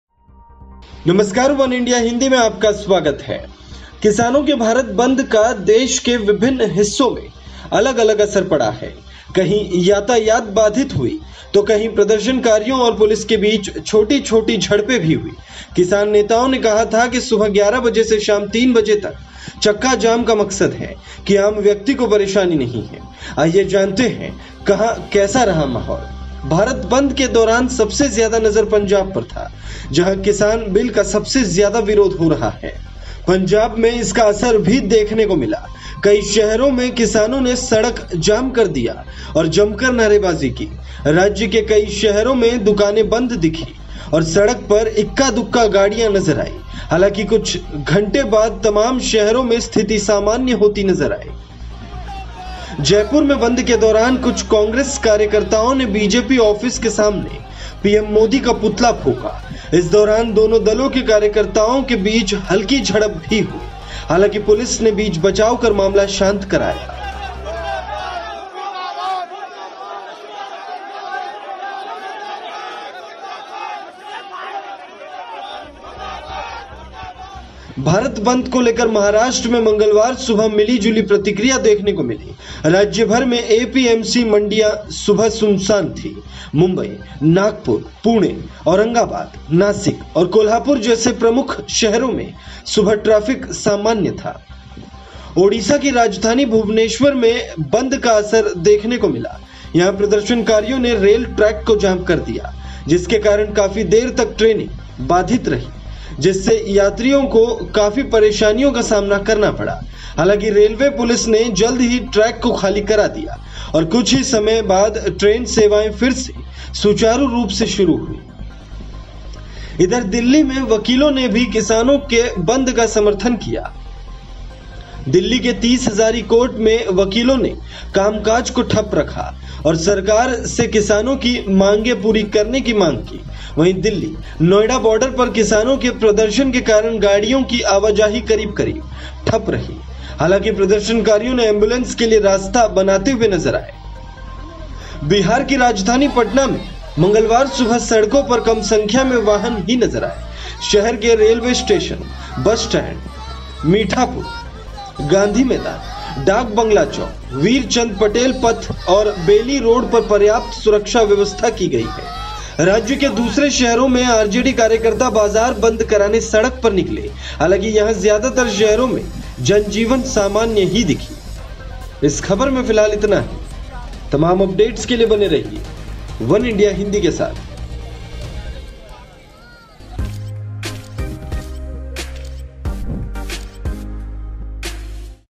Bharat Bandh: देशभर में किसानों के इस आंदोलन का कैसा रहा असर ? | वनइंडिया हिंदी
A nationwide strike (Bharat Bandh) on Tuesday called by agitating farmer unions to press for repeal of the Centre's agri laws does not seem to have any major impact in the country with banks and markets functioning as usual.

किसानों के भारत बंद का देश के विभिन्न हिस्सों में अलग-अलग असर पड़ा है। कहीं यातायात बाधित हुई तो कहीं प्रदर्शनकारियों और पुलिस के बीच छोटी-छोटी झड़पें भी हुईं। किसान नेताओं ने कहा था कि सुबह 11 बजे से शाम 3 बजे तक चक्का जाम का मकसद है कि आम व्यक्ति को परेशानी नहीं है।